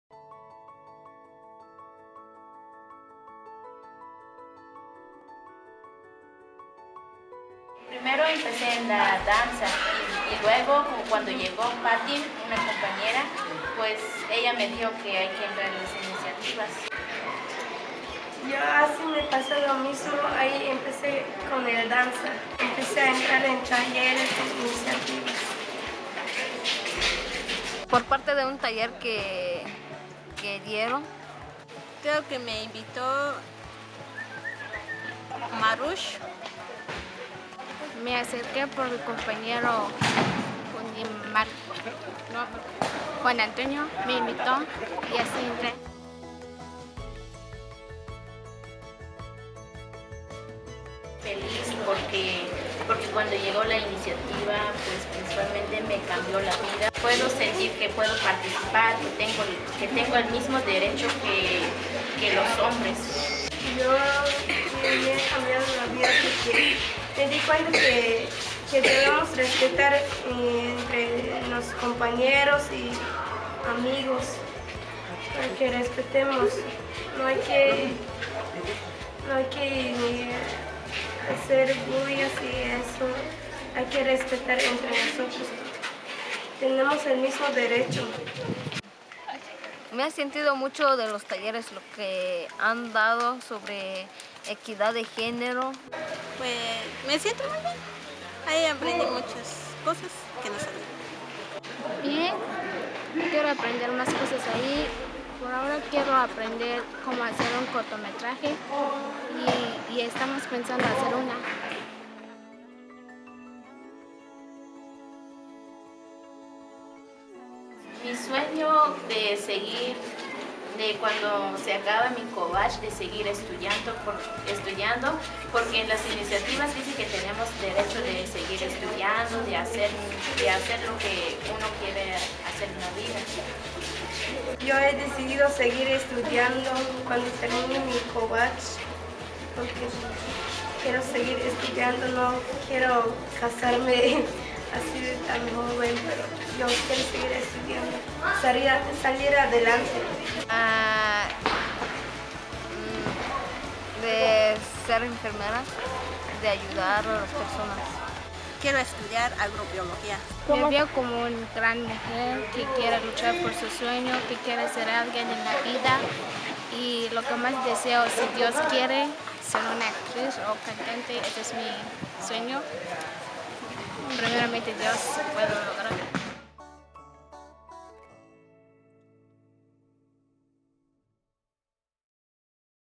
Iniciativas Juveniles de Ch'ieltik
Las Iniciativas Juveniles son uno de los programas que trabajamos en  Ch'ieltik, en el apoyamos las ideas y proyectos que las y los jóvenes tienen  para mejorar sus comunidades.
Ellas y ellos nos cuentan sus experiencias desde que son parte de este proyecto.